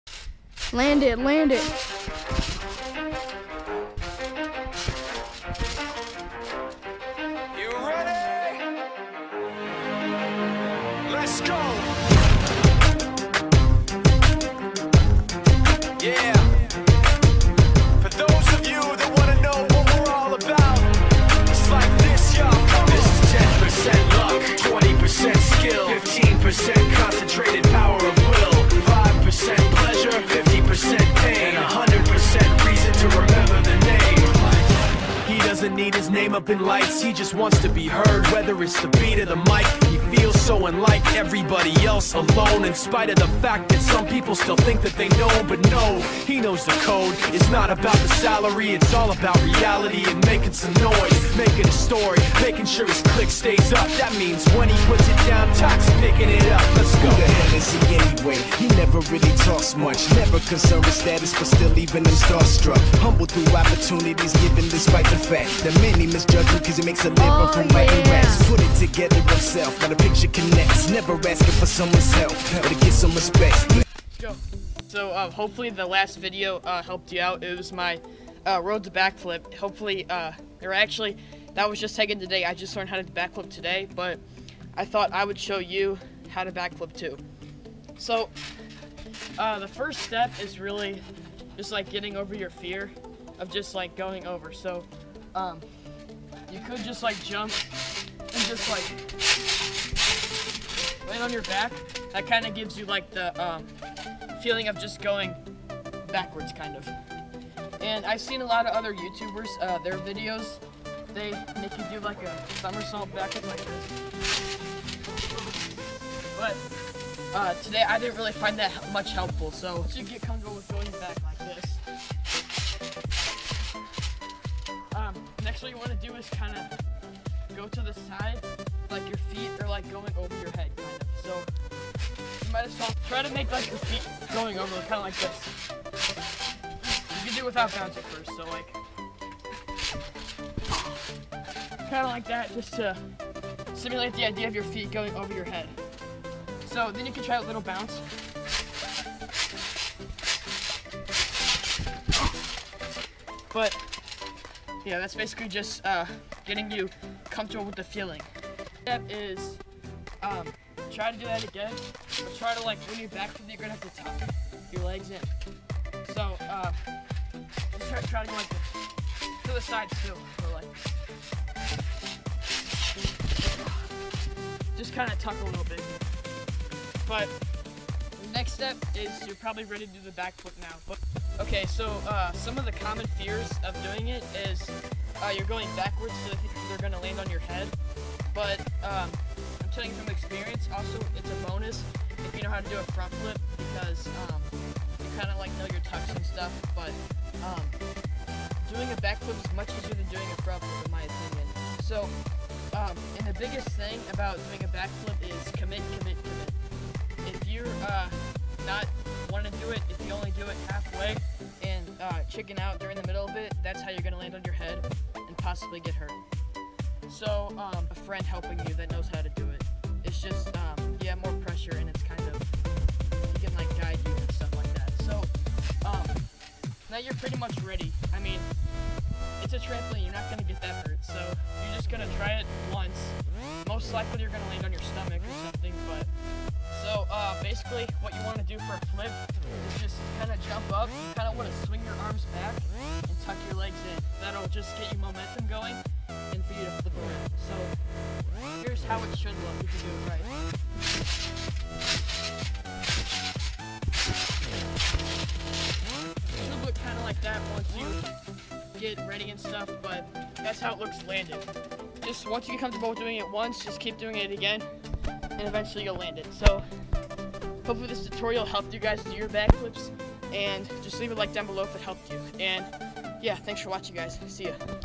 ROAD TO BACK FLIP!!/How to do a Back Flip- Easiest Method
In this video I will be showing you how to do a backflip, the easiest way.